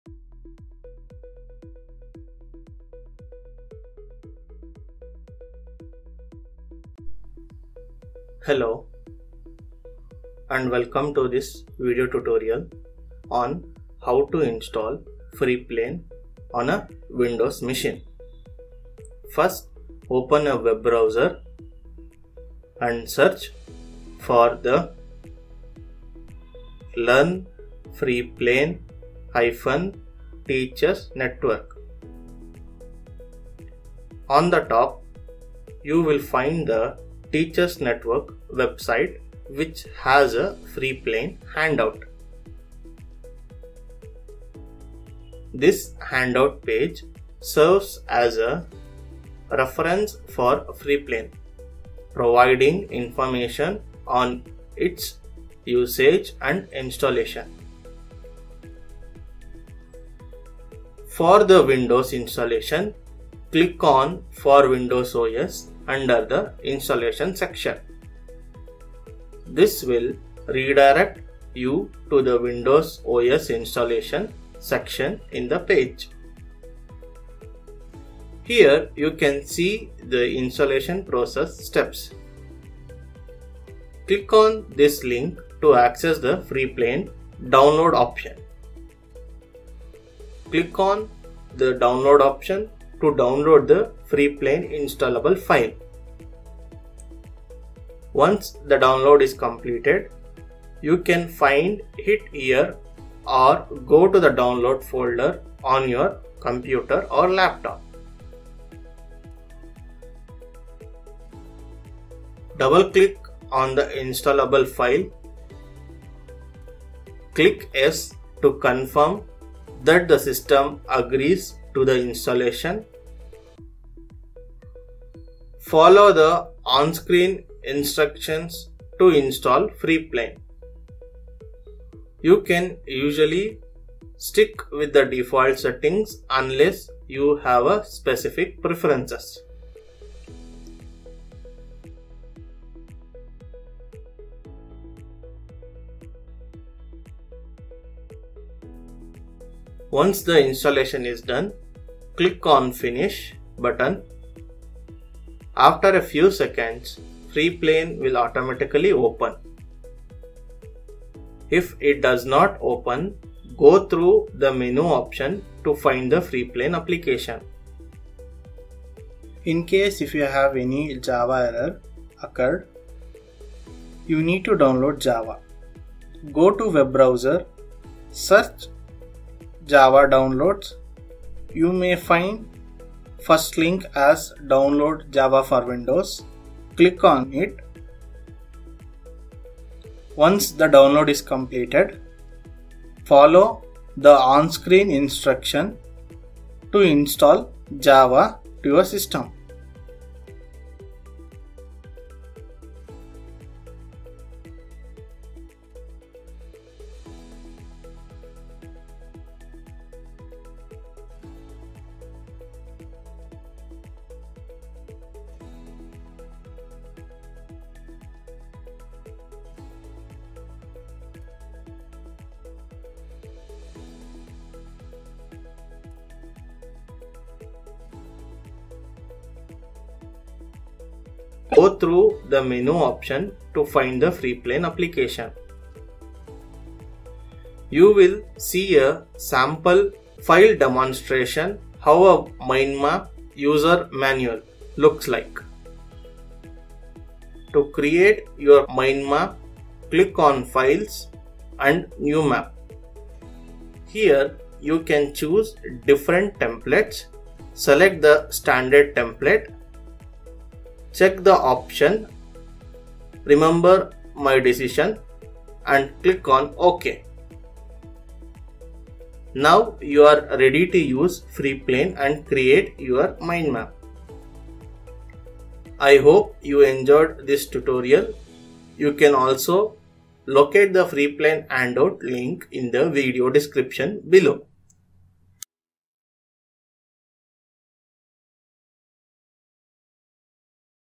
Freeplane (concept mapping) tool installation on Microsoft windows
This video tutorial helps you install Freeplane on Windows 10, 11, and older versions where Java installation is required. You can also refer to the "Learn Freeplane Handout" This handout will guide you on using Freeplane to create concept maps.

If you're using an older version of Windows, you'll need to install Java to work with Freeplane.